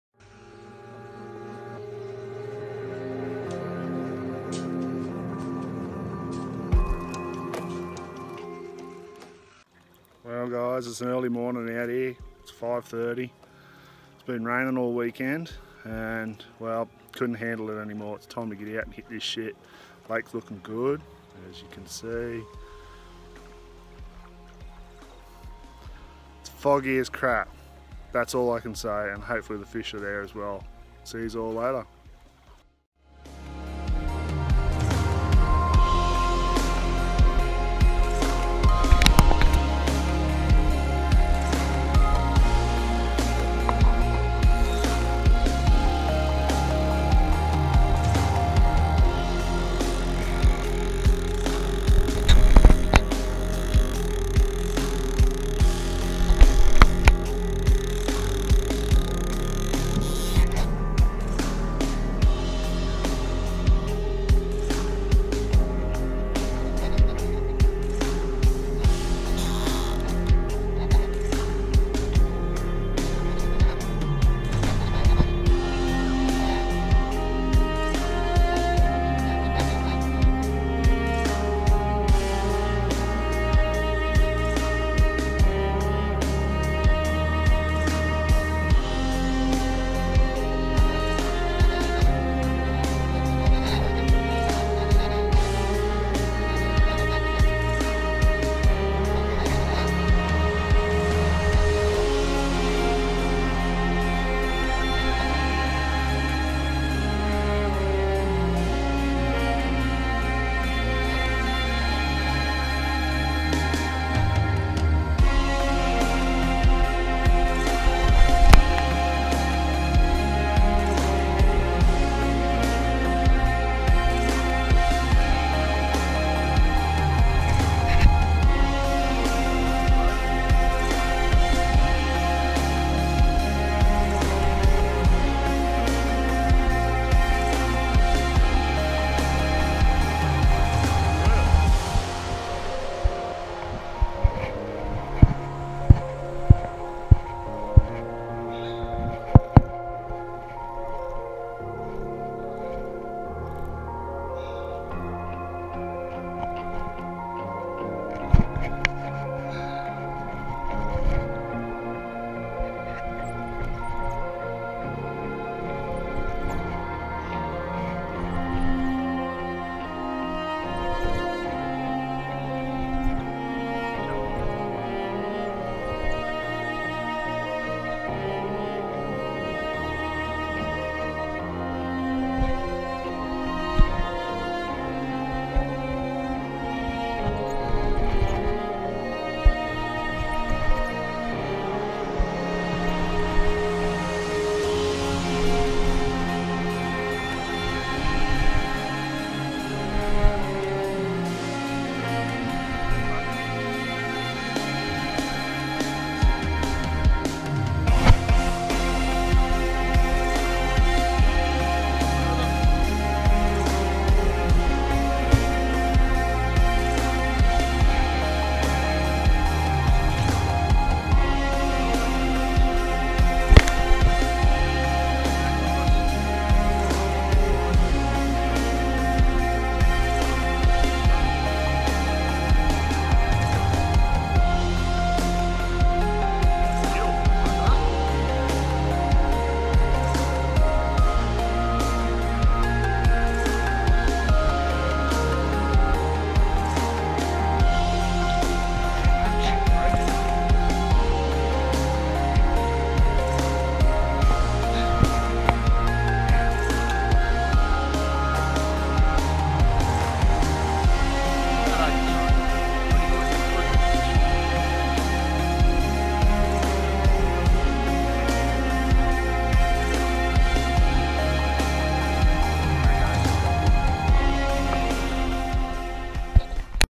Lake Mac Mulloway
Its Monday The 10th of February 2020, and ive just spent the weekend
watching tv and being cooped up in the house due to the ECL, but i caught a break and decided to hit the Lake Macquarie for a fish, even knowing it was like a soup bowl of muddy water i went anway and it was worth the early morning, heres one of the clips hope you all enjoy it